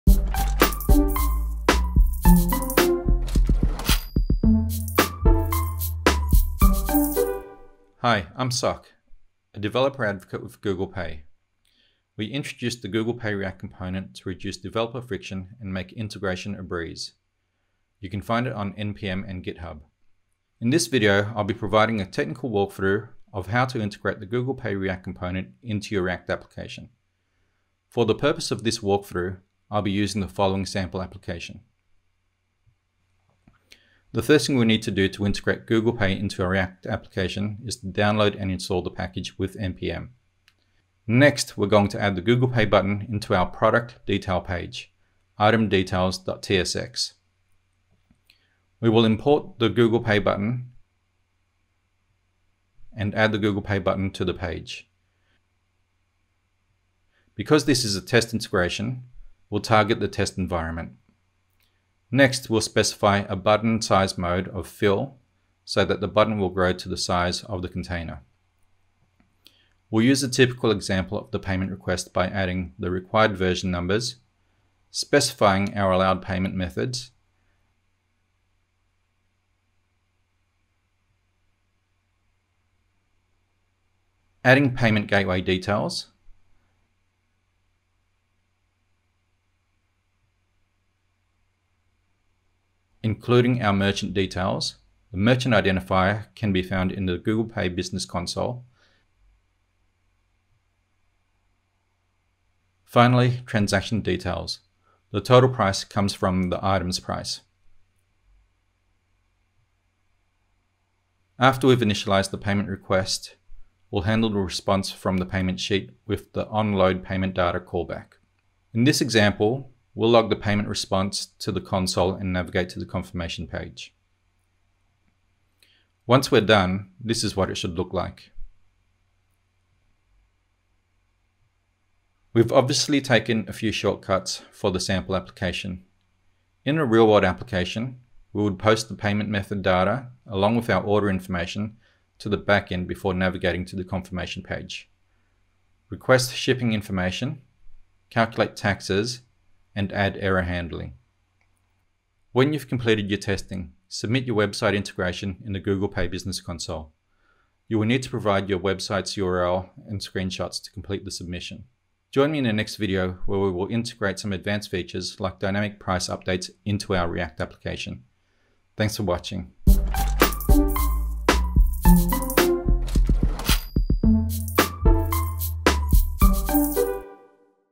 How to integrate Google Pay in React - Payments DevBytes
The Google Pay React component makes integrating Google Pay into your React app easier than ever. Watch a step-by-step tutorial on how to integrate the Google Pay React component in React.

product: Google Pay - gPay; fullname: Soc Sieng;